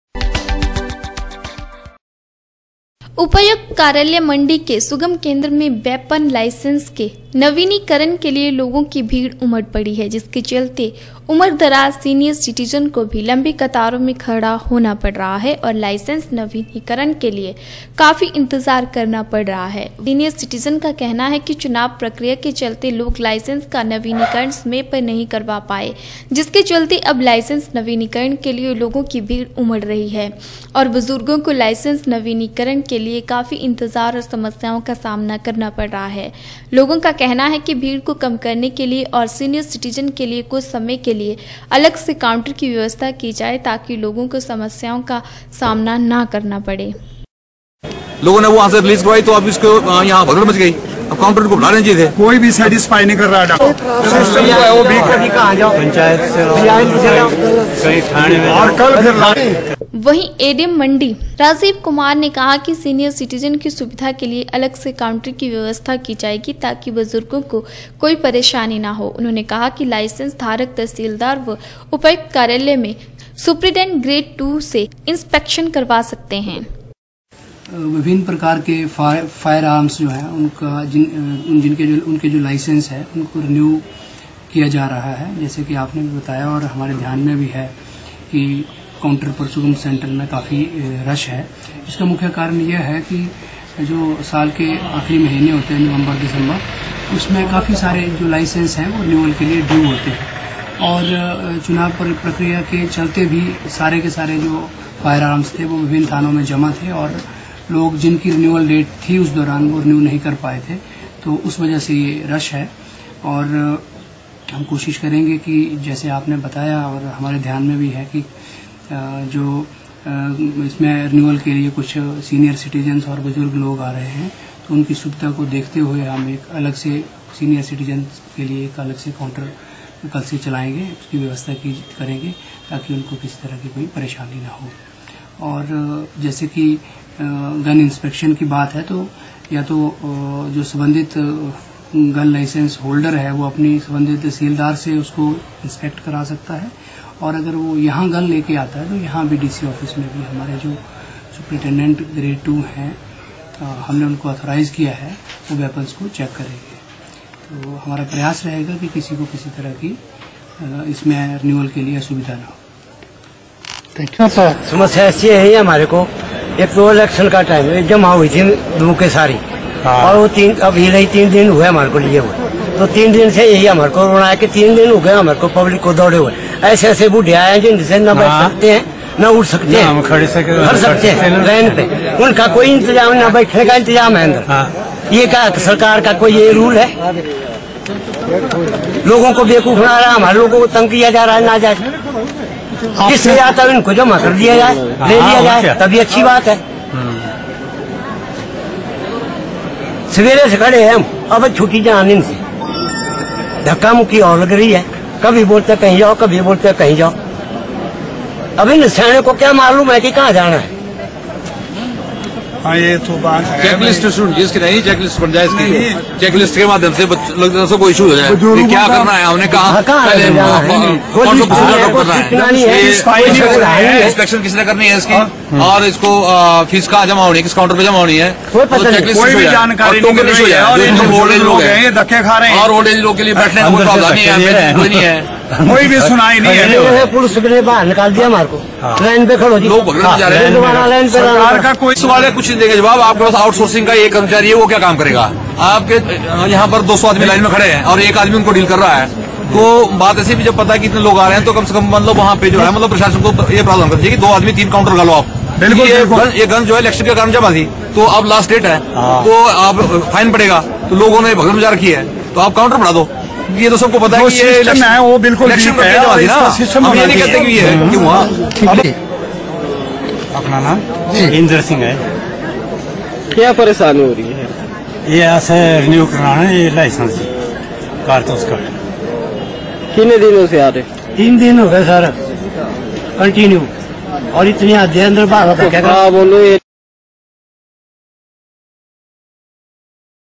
सुगम केंद्र में वैपन लाईसैंस के नवीनीकरण के लिये लोगों की भीड़ उमड़ पडी है
उपायुक्त कार्यालय मंडी के सुगम केंद्र में वैपन लाईसैंस के नवीनीकरण के लिये लोगों की भीड़ उमड़ पडी है। जिसके चलते उम्रदराज सीनियर सीटीजन को भी लबंी कतारों में खडा़ होना पड रहा है और लाईसैंस नवीनीकरण के लिये काफी इंतजार करना पड रहा है। वही ंए डी मडी राजीव कुमार ने कहा कि सीनियर सीटिजन को समस्याओं का सामना ना करना पडे इसके लिये सुगम सैंटर में सीनियर सीटिजन की सुविधा के लिये अलग काउटर की व्यवस्था की जायेगी तांकि सीनियर सीटिजन को लंबा इंतजार ना करना पडे।